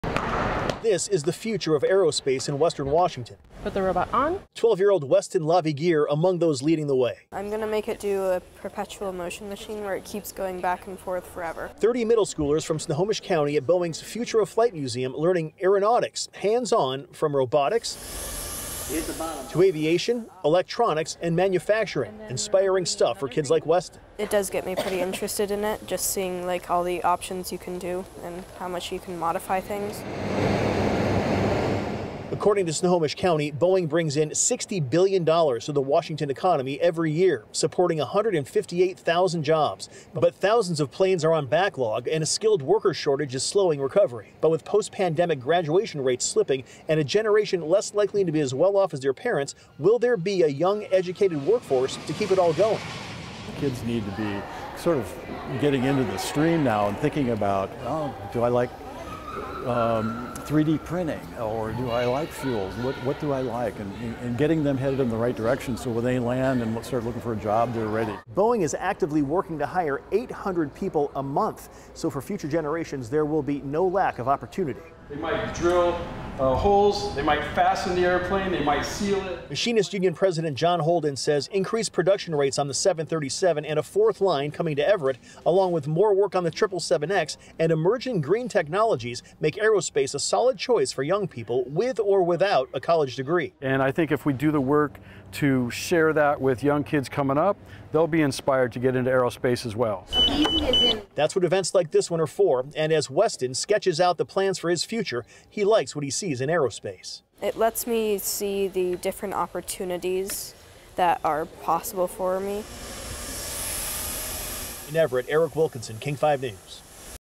Snohomish County hopes to recruit young minds to aerospace
One by one, the future of aerospace in western Washington filed into Boeing's Future of Flight Museum on Wednesday.

Among those leading the way was 12-year-old Weston Lavigueure.

Thirty middle schoolers from Snohomish County visited the museum for a hands-on learning session involving aeronautics, robotics, aviation, electronics and manufacturing.

The eighth graders flew drones, practiced welding via virtual reality and wrote code that directed the movement of robots.